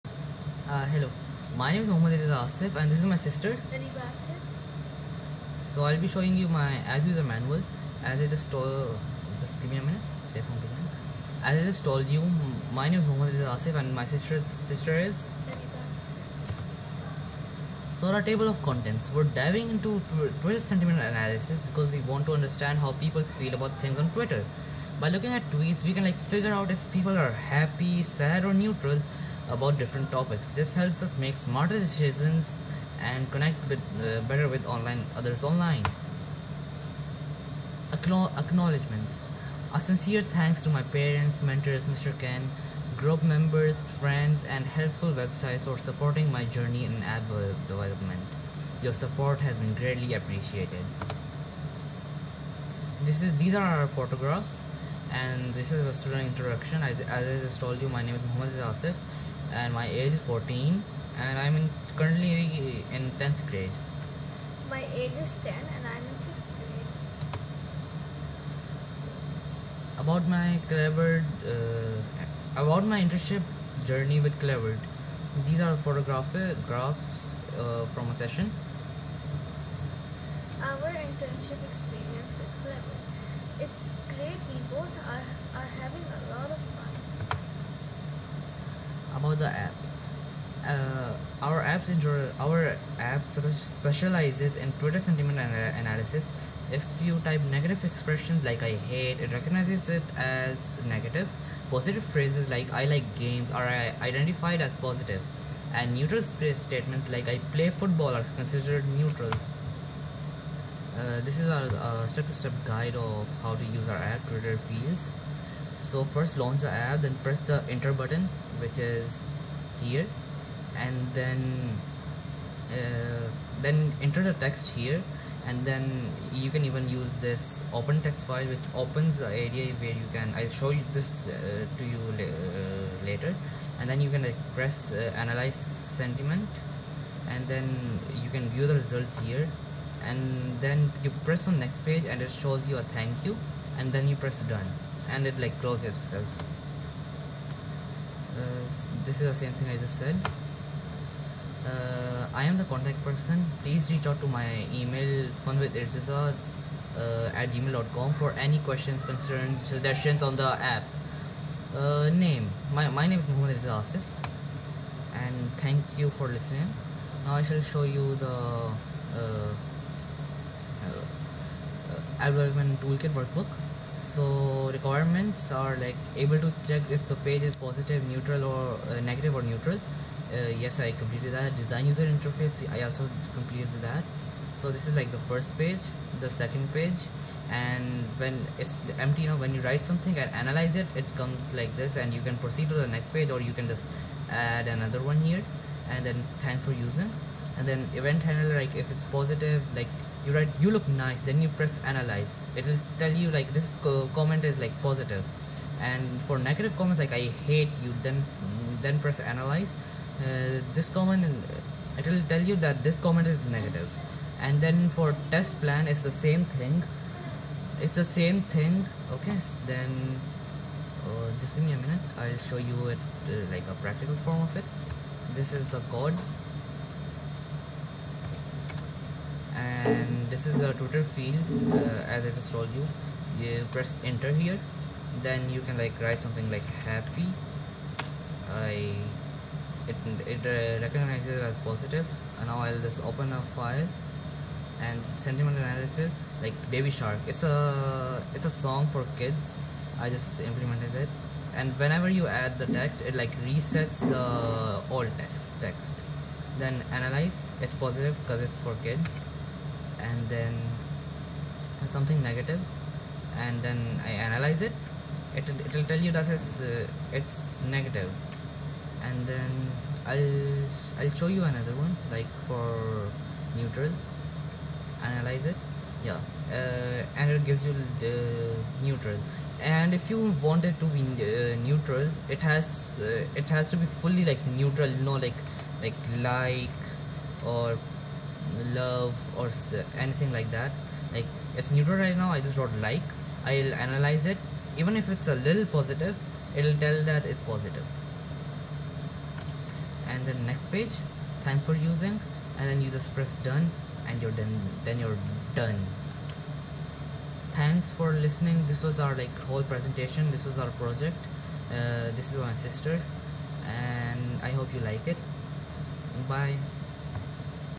TwitterFeels | Advanced Internship June 23 cohort | Clevered
Muhammad Irteza Asif and Areeba Asif created a project named TwitterFeels. "TwitterFeels" is a sentiment analysis tool for analyzing tweets and text data to determine sentiment (positive, negative, or neutral).